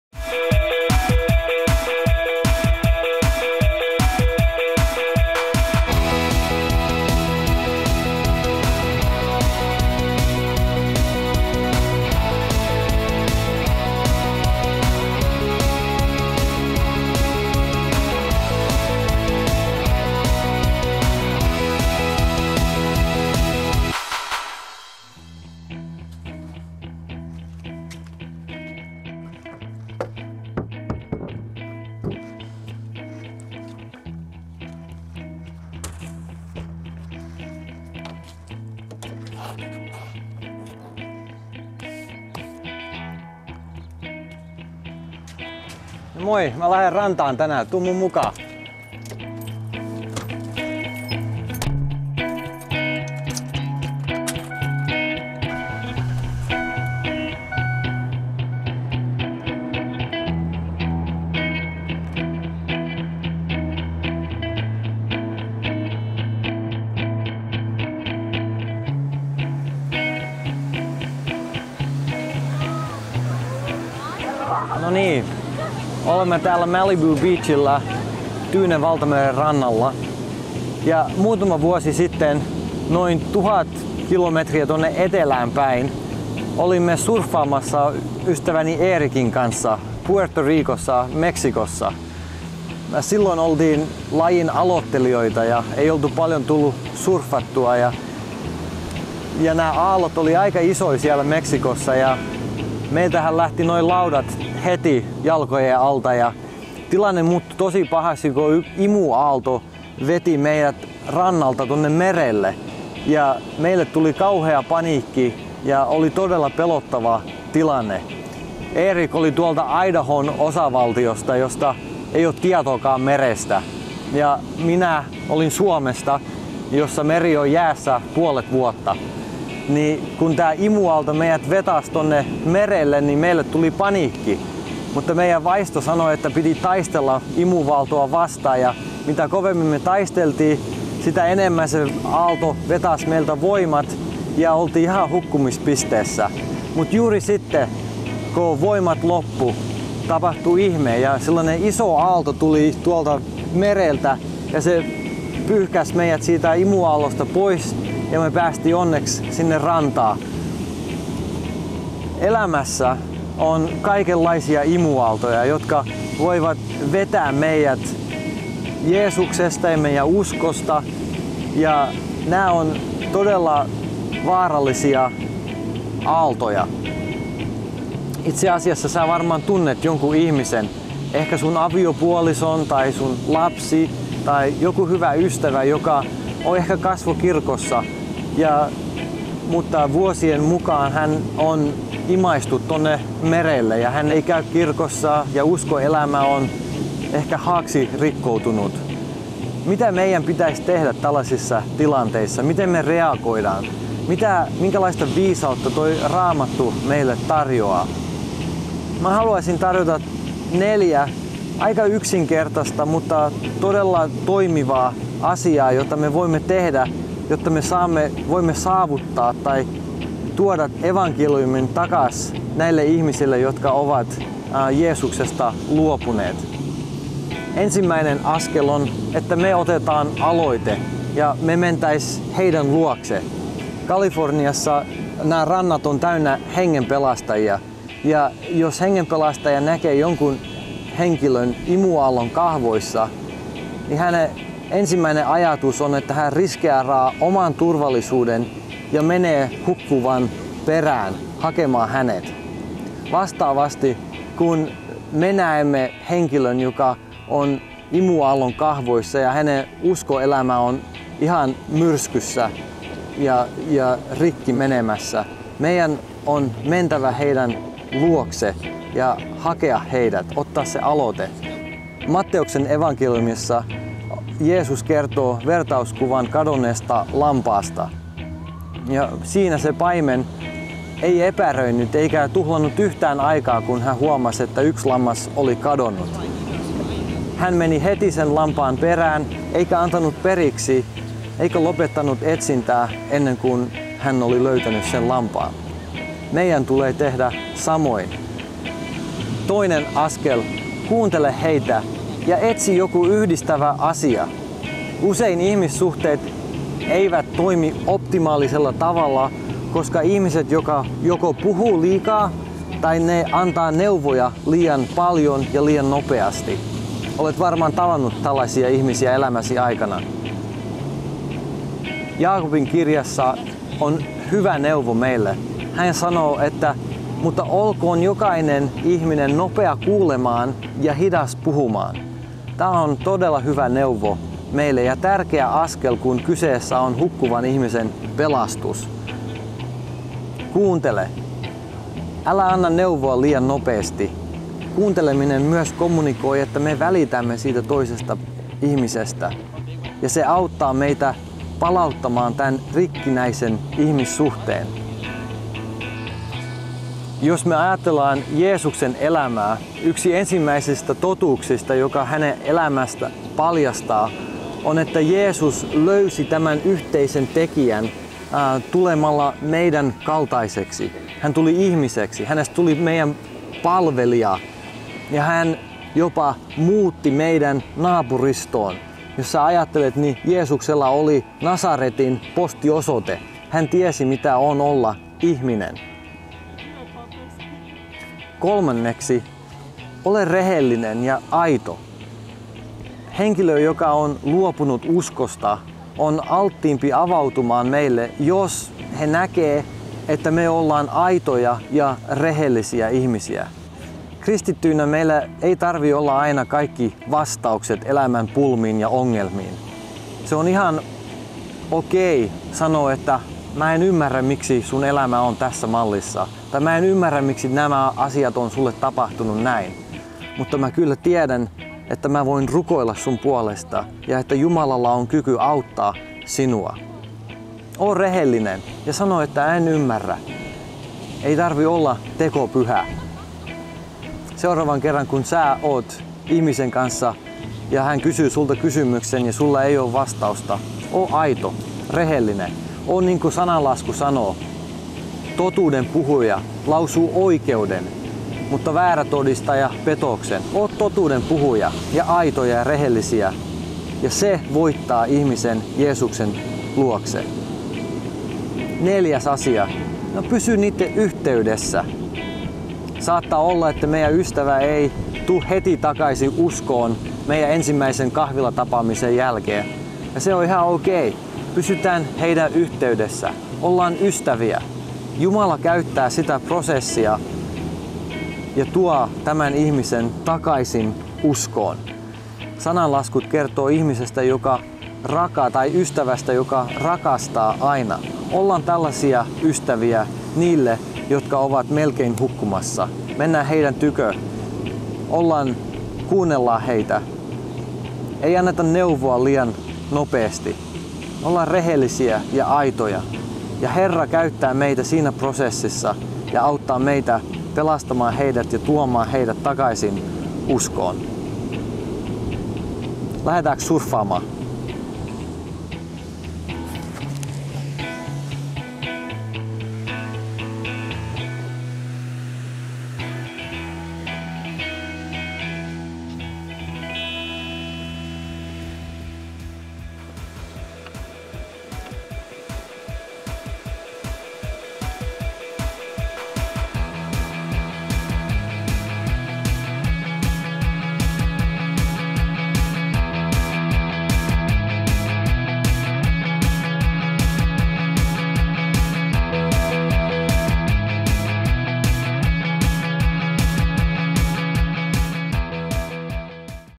Cross Training 03 - Takaisin elämään
Jakso 03. Tässä jaksossa Joel Perttula opettaa kuinka voimme kohdata uskosta ja seurakuntaelämästä etääntyneitä ystäviämme ja johdattaa heidät takaisin Jumalan ja seurakunnan yhteyteen.